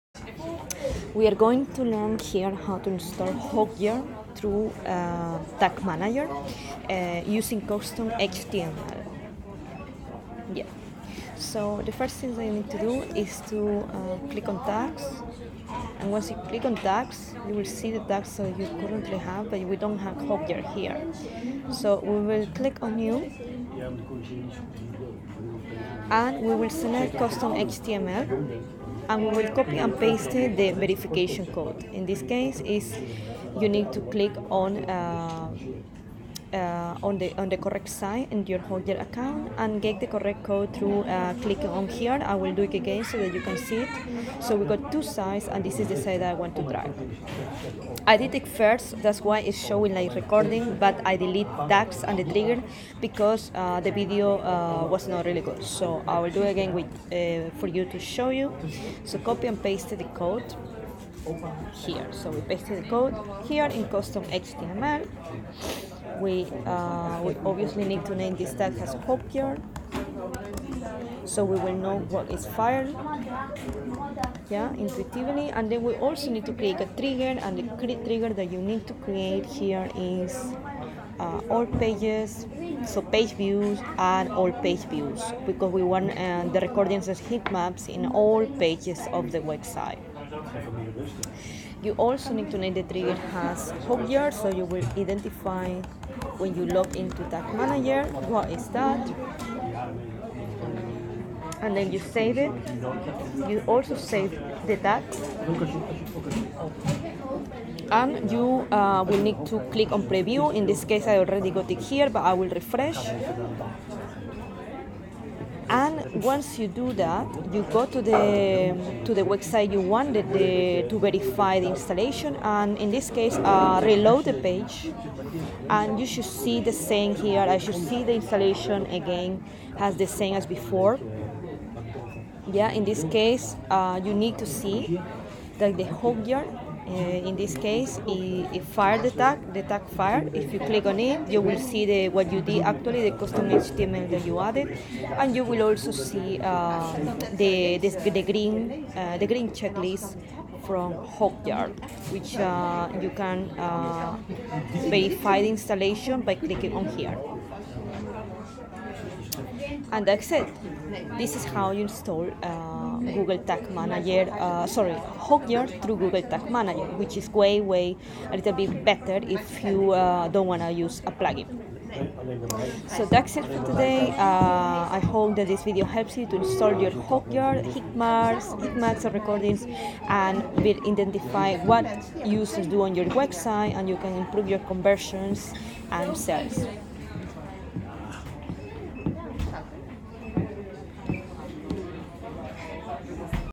Installing Hotjar with Google tag manager
In this video, you will learn how easy is to install Hotjar with tag manager in a few easy steps!
Tracking what your users do through heatmaps and recording with Hotjar will help you to identify weak points of your website for improvement but installing the Hotjar tracking code is the first thing you need to get right.
Learn here how to install it with Google tag manager!